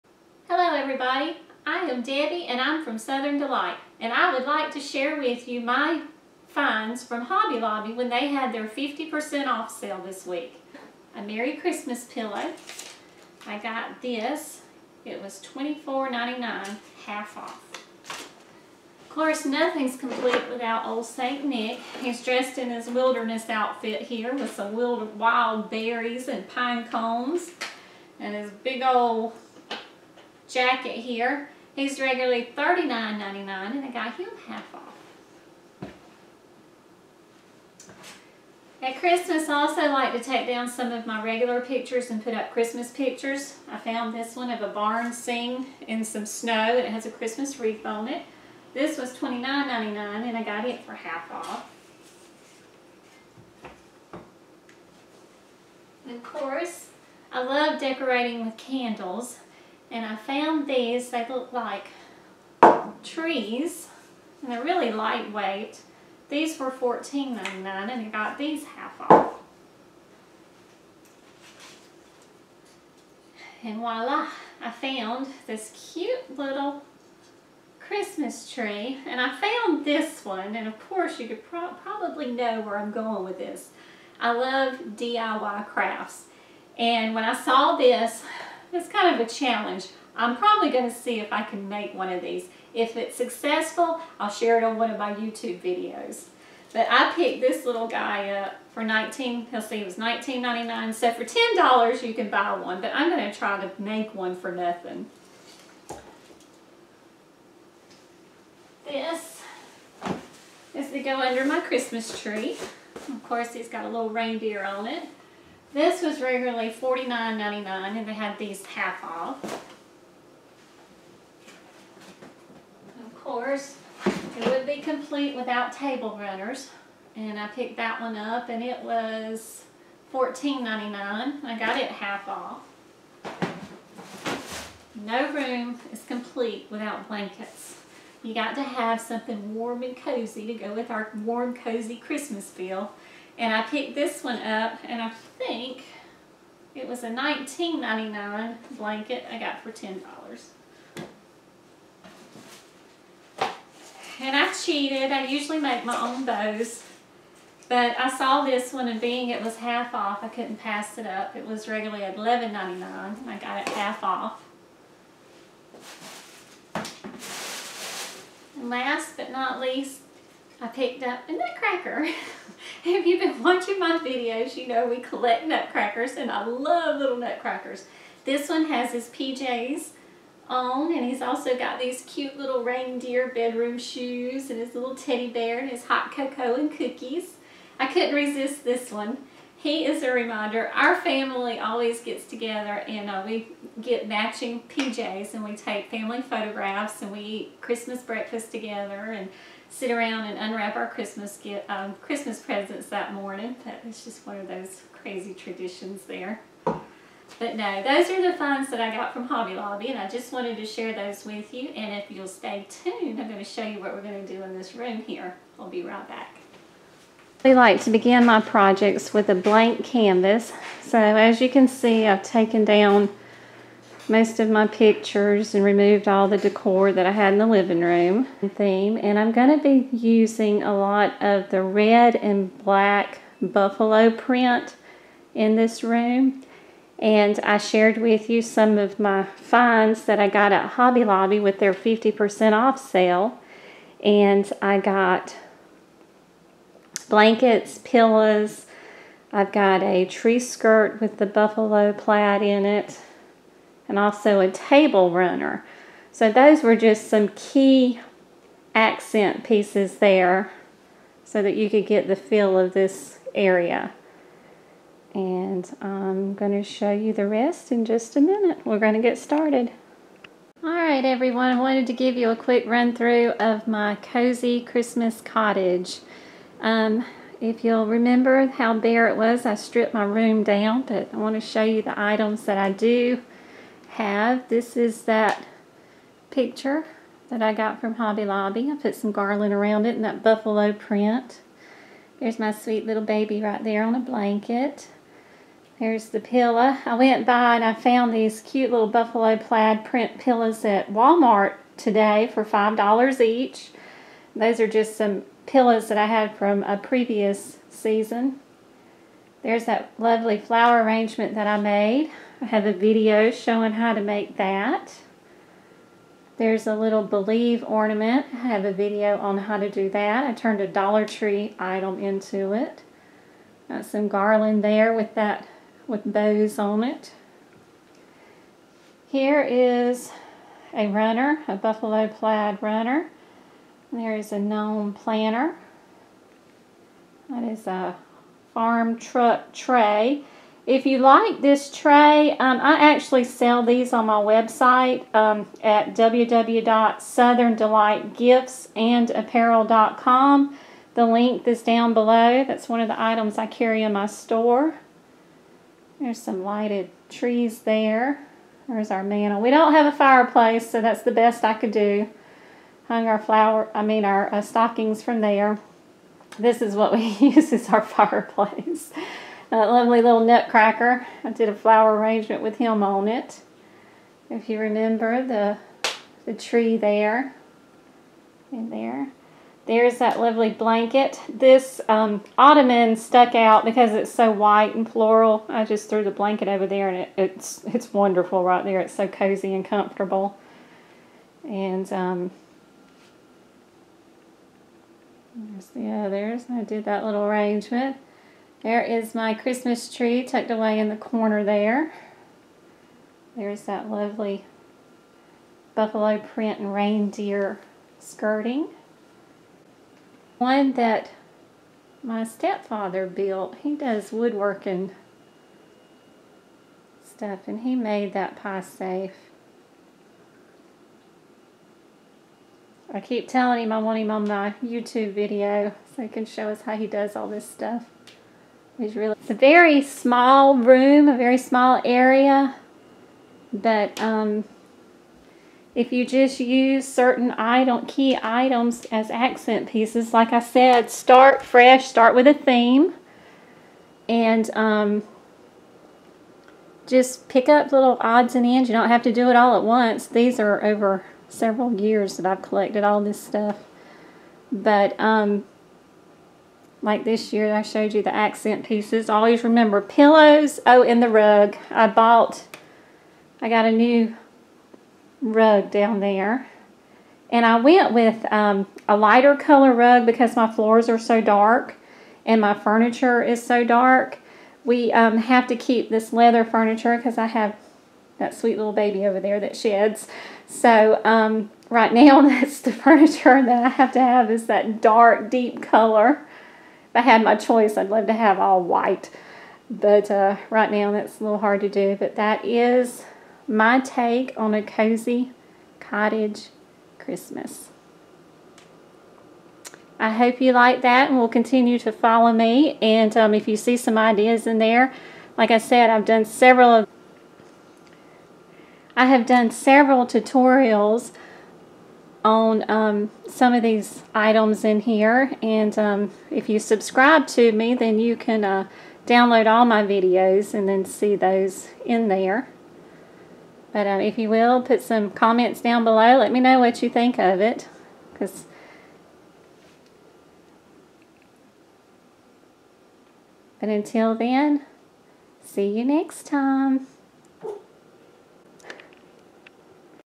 Christmas Home Tour;Warm cozy christmas cottage; holiday decor;dollar tree DIY
Home Decor/Living Room transformed into a Cozy Christmas Cottage using items from Hobby Lobby. Cottage/Buffalo  Plaid Print Theme.
https:\\youtu.be\EJqxFHGzzWc (ornament)
https:\\youtu.be\SeR13qz4rlU. (flower Arrangement)